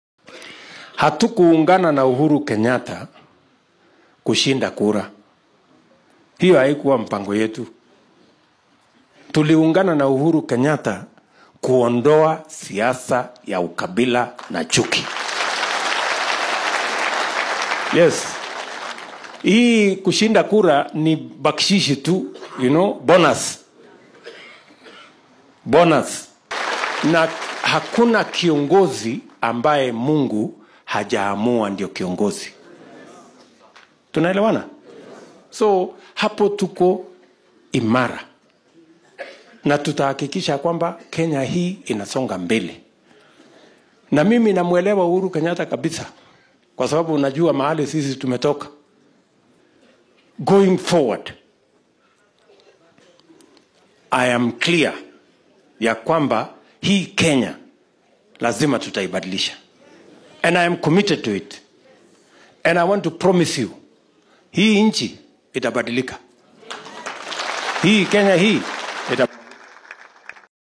Lose or win, I don’t want Kenyans to kill each-Ruto
Deputy President William Ruto says he will rather forgo power than see Kenyans fight over him or any other politician, adding that if it means people have to fight for him to get power, then he better forgo that power.